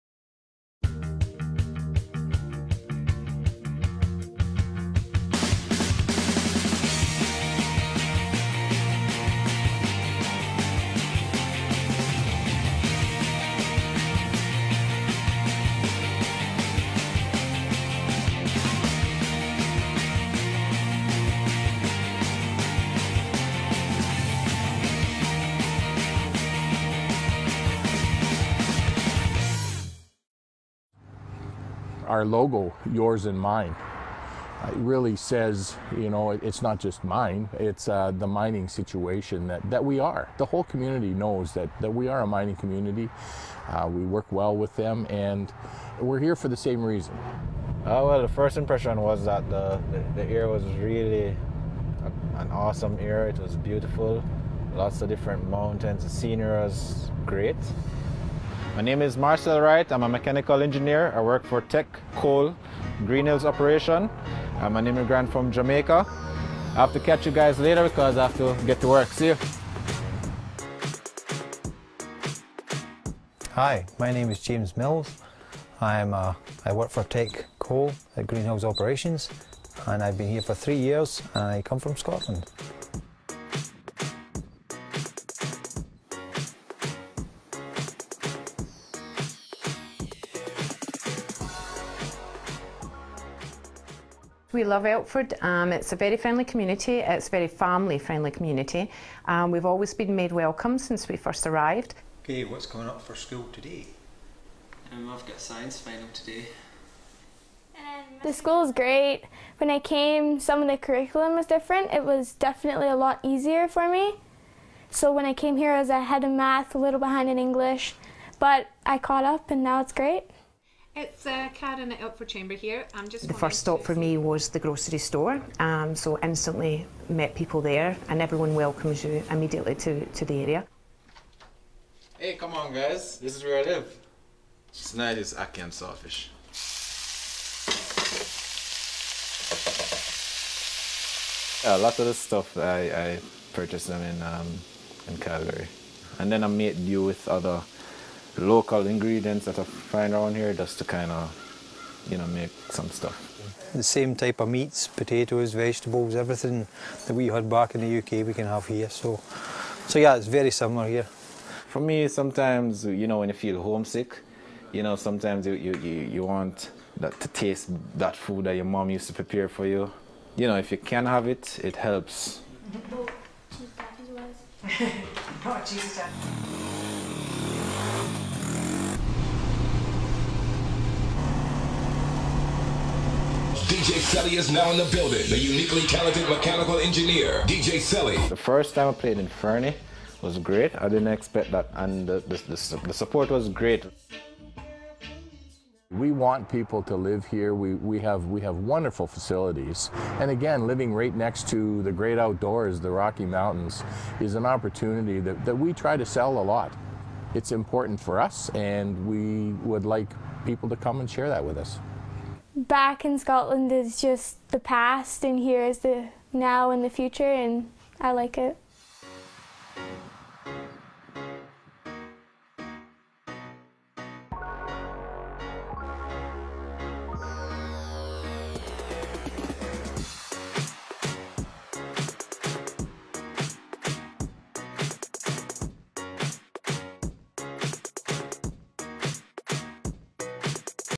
This Home of Mine - Immigrant Perspectives from the Elk Valley
Sparwood and Elkford are both located in southern British Columbia (Fernie is approximately 22 min away by car, while Calgary is 2 h 42 min). They are both typical examples of small mining communities in Canada where the local mine is the primary employer. This video features perspectives from immigrants who have settled in the region to work at the local mine.